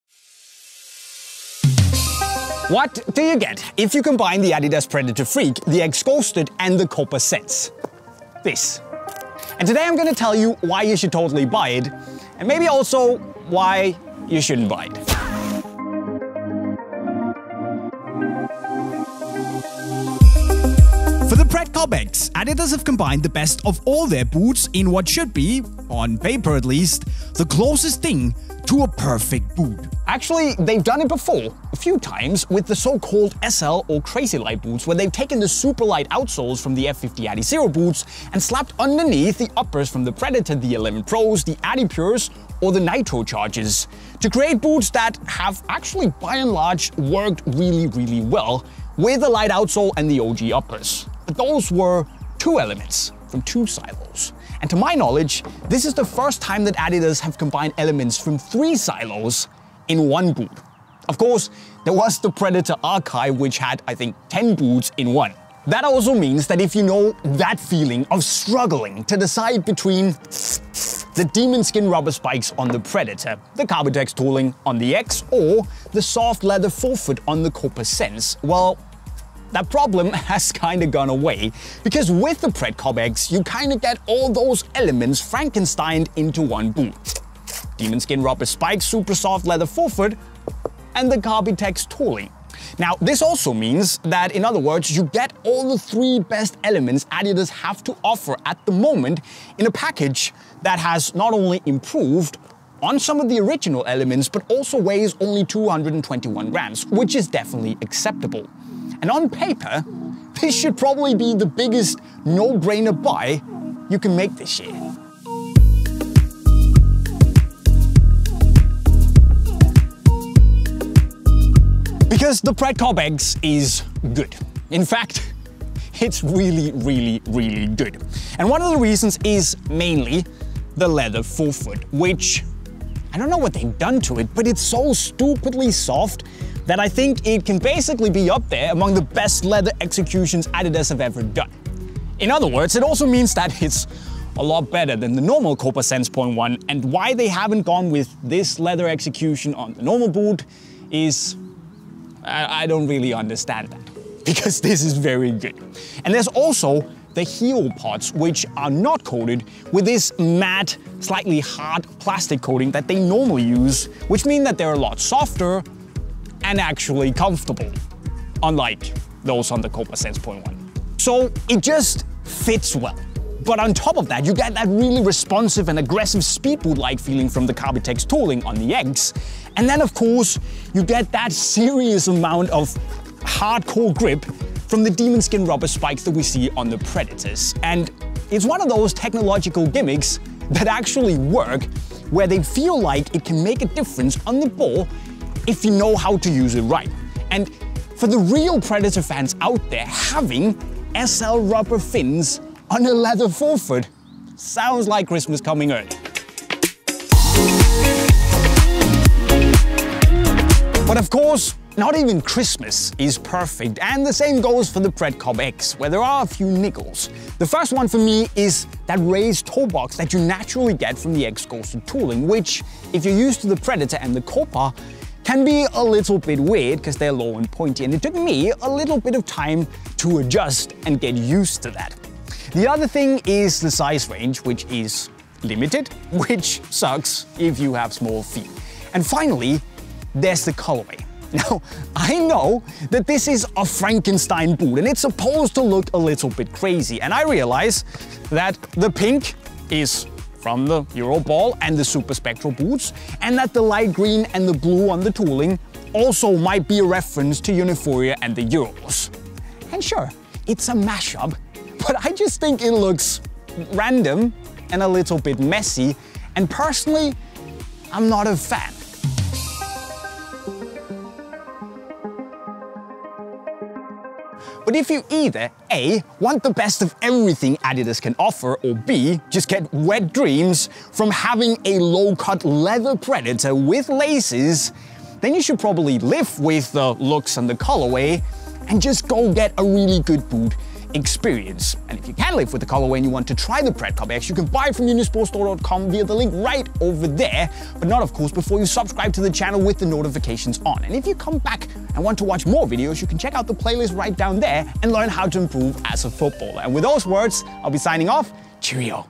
Did adidas make the PERFECT hybrid football boot?
adidas PredCopX Play Test and review.

In today's review of the adidas PredCopX football boots, JayMike takes a look at the Frankenstein hybrid football boots which is a mashup between the adidas Predator Freak, the adidas Copa Sense and the adidas X Ghosted - hence the PredCopX name - in order to see if it's as good as adidas suggest and whether it might be the perfect football boots for you to buy.

As per usual in our play test and review videos, JayMike will tell you everything you need to know about the tech behind the adidas PredCopX as well as how it fits and feels in terms of comfort and of course, how it performs on the pitch with the Demonskin Predator rubber fins, the soft Copa k-leather forefoot and the Carbitex outsole from the X. Is this the PERFECT adidas hybrid football boot? Well, check out the review and find out for yourself.